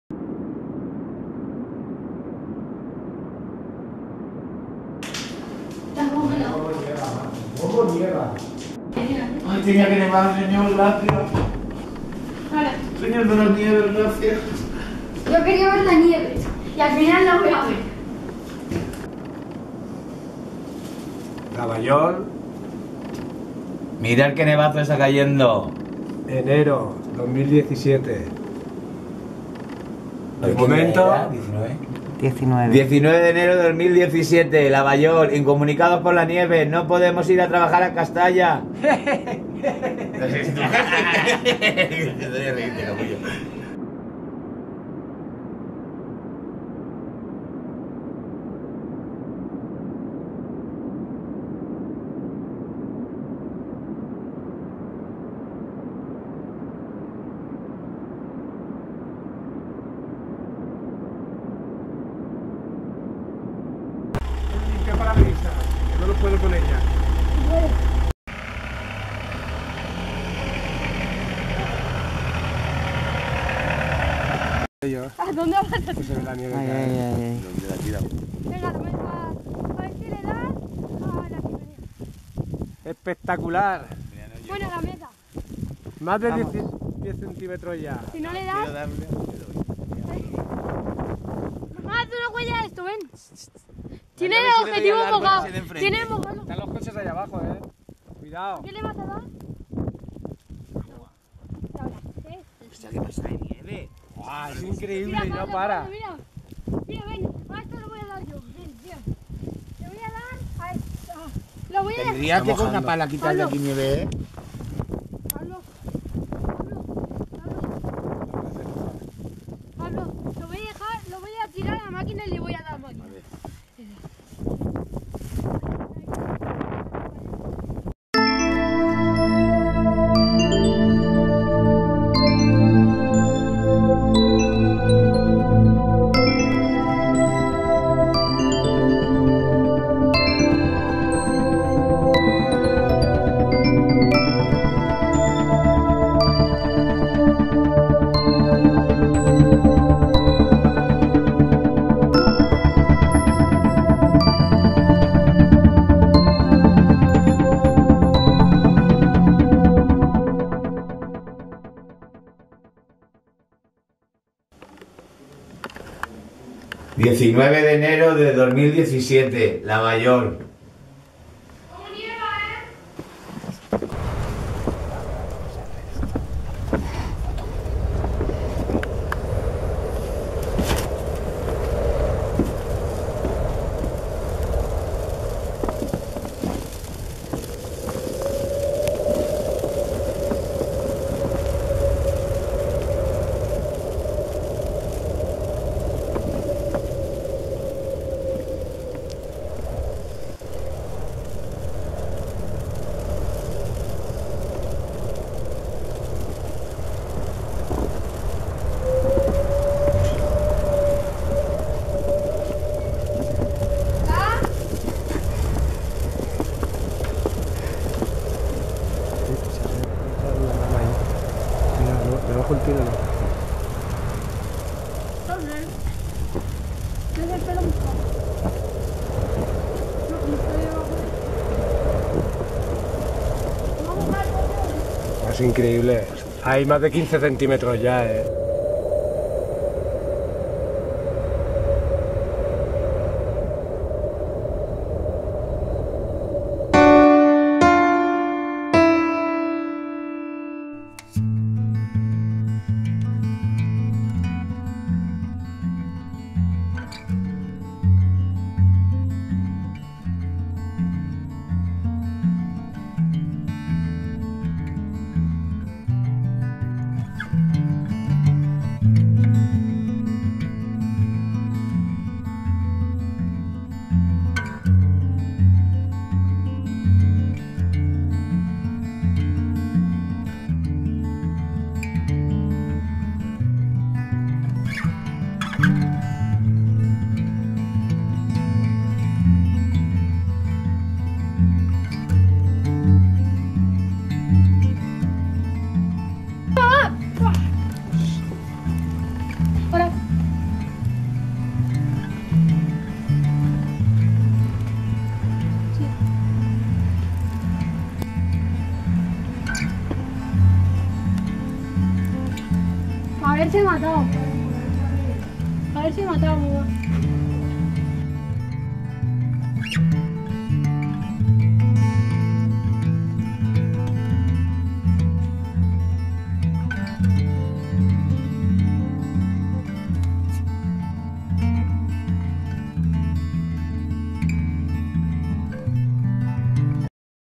Gran nevada en L'Avaiol (Alicante, Paisaje Protegido Maigmó-Cid)
Gloriosa nevada la del 19 de enero de 2017 en L' Avaiol, en el corazón del Paisaje Protegido Maigmó-Cid (Petrer, Alicante), con hasta 25 centímetros de nieve que acabaron posándose.

Un espectáculo, sin duda. Se desprendió una gran rama del enorme pino que hay a la entrada de la finca de la asociación que pudo causar un susto y algunos miembros del colectivo perdieron incomunicados un día de trabajo, pero a cambio pasaron una noche y un día inolvidables...